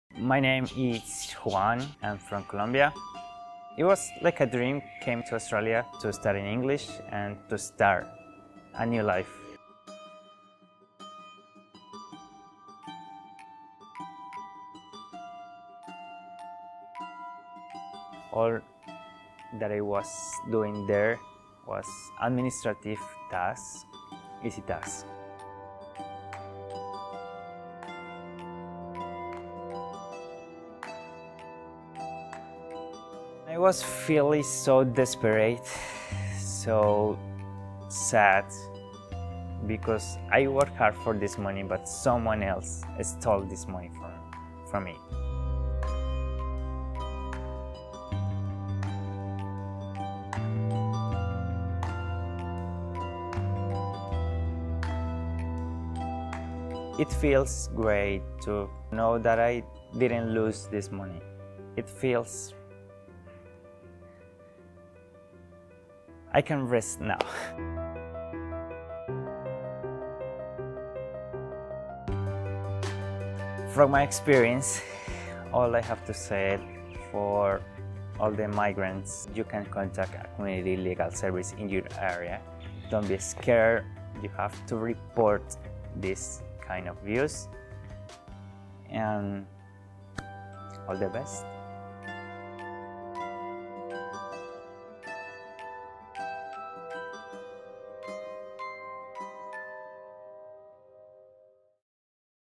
후안의 이야기
콜롬비아에서 온 후안은 불법 인턴십 알선업체에 돈을 지불하고 근무지에서 무급으로 일을 하며, 약속 받았던 OJT(현장실습교육)도 받지 못했습니다.
어떻게 후안이 법률구조센터의 도움을 받아 임금을 돌려 받을 수 있었는지 자세한 내용을
살펴보세요.